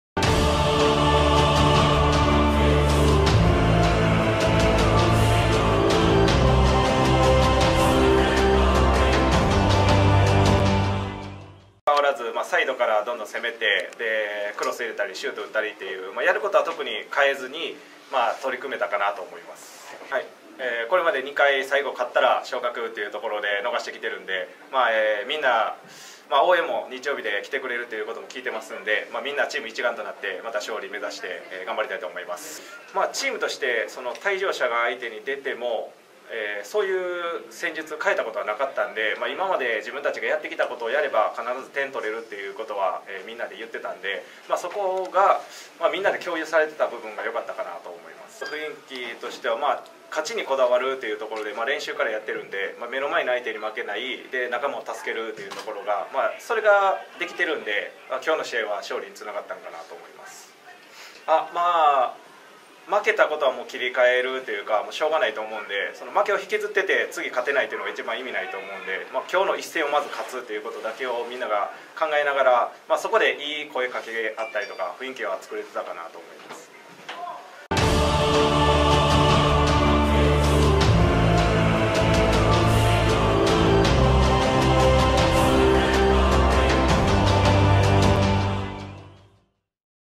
地域CL2021 おこしやす京都AC MF清水良平 決勝ラウンド大会2日目 FC徳島戦コメント
FC徳島 vs おこしやす京都 決勝ラウンド大会2日目ダイジェスト